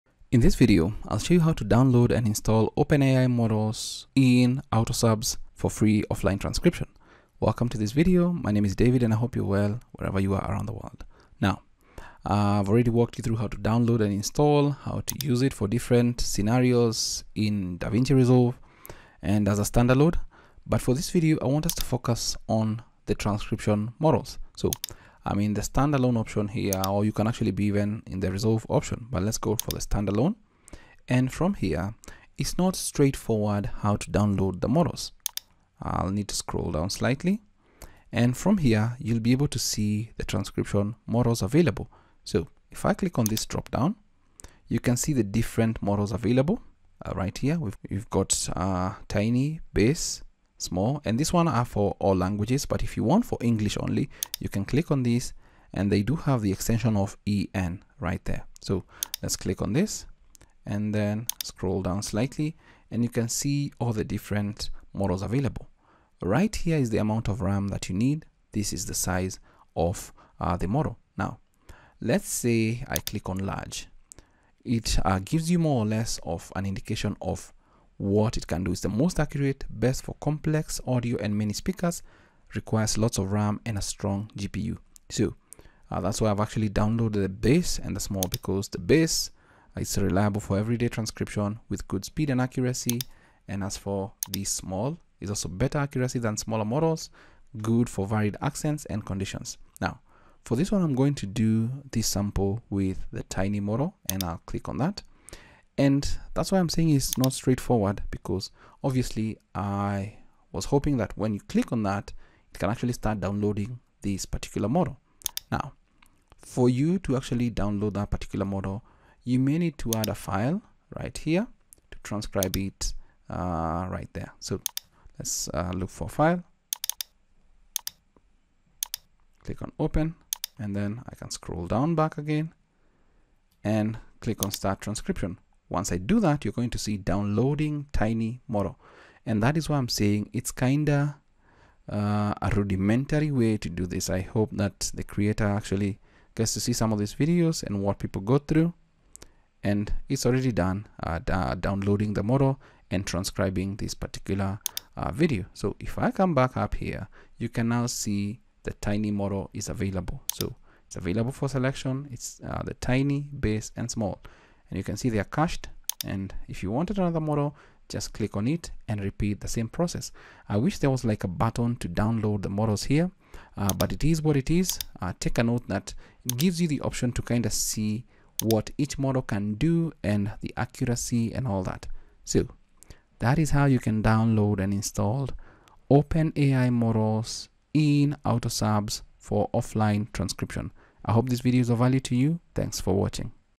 How to Download and Install Open AI Whisper Models in AutoSubs for Offline Transcription
In this video, I'll show you How to Download and Install Open AI Whisper Models in AutoSubs for Offline Transcription.